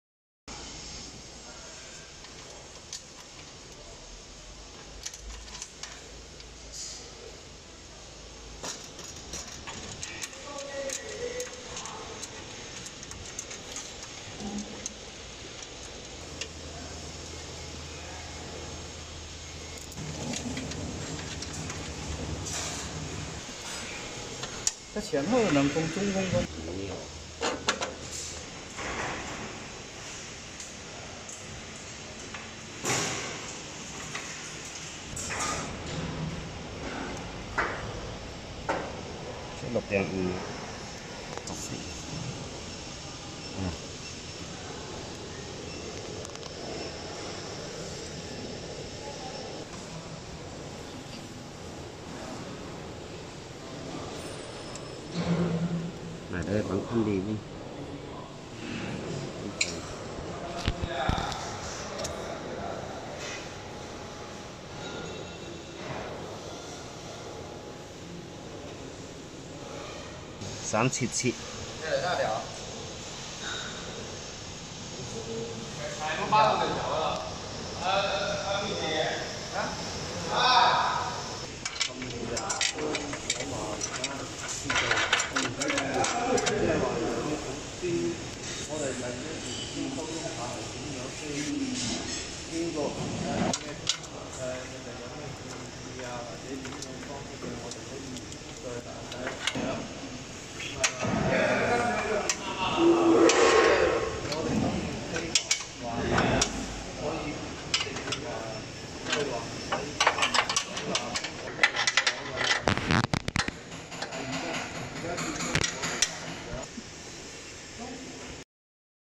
How to connect the conveyor with flowpack-mask packing machine
Mask packing machine ,KN95 mask packing machine -How to setting the eye mark